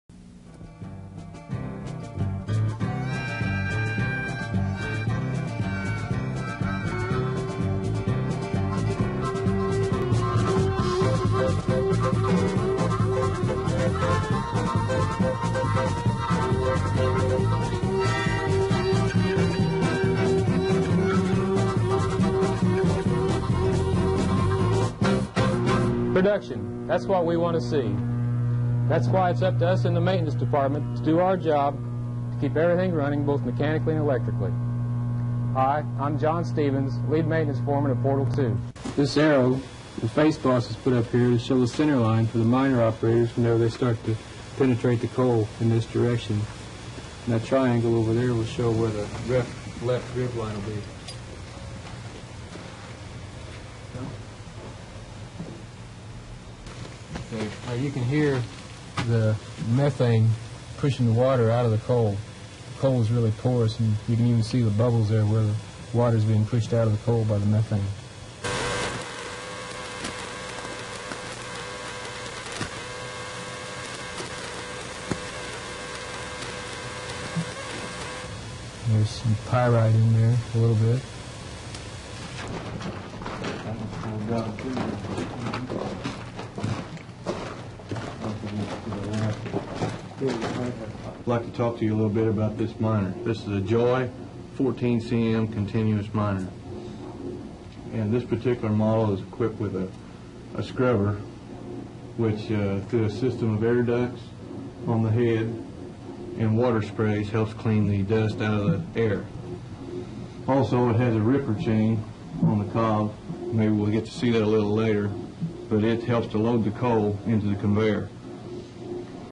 Amax Coal's Wabash Underground Mine
This is a brief walk through Amax Coal Company's Wabash Underground mine in 1984.  John Stevens explains how methane escapes from the coal face and continuous miners are used for coal extration.